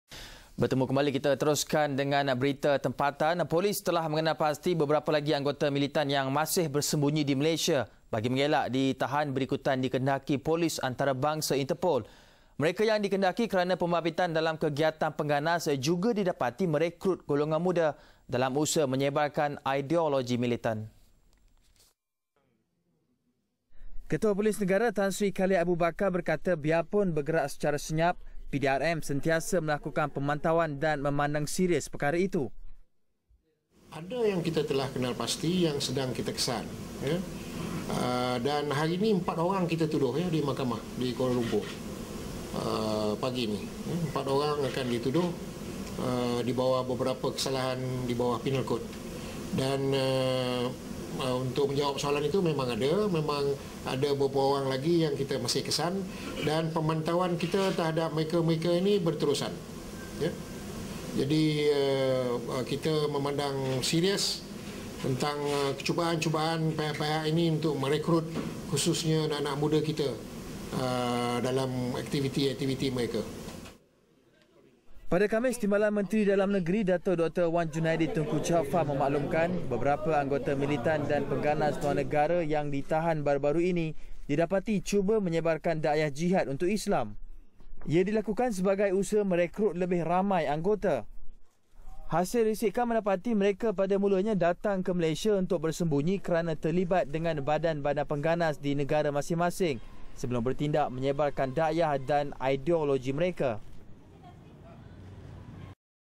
Malaysia jadi lokasi persembunyian anggota militan
Polis telah mengenal pasti beberapa lagi anggota militan yang masih bersembunyi di Malaysia bagi mengelak ditahan berikutan dikehendaki polis antarabangsa (INTERPOL).

Mereka yang dikehendaki kerana pembabitan dalam kegiatan pengganas juga didapati merekrut golongan muda dalam usaha menyebarkan ideologi militant.